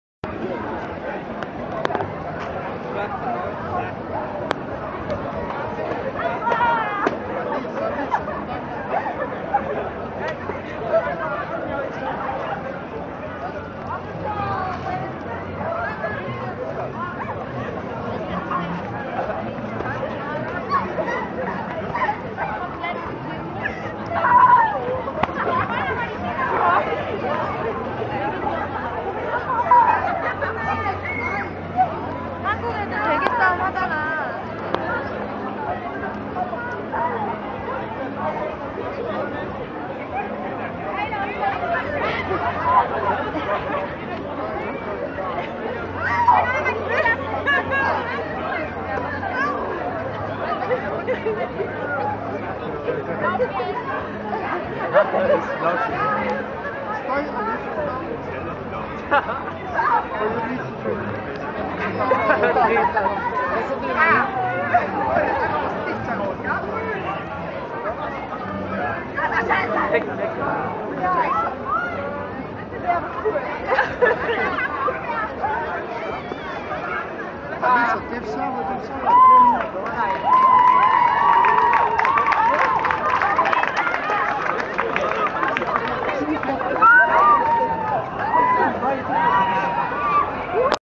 Pillow Fight Flash Mob.3GP
A pillow fight ala  flash mob in front of Galleria Frankfurt, Spring 2012.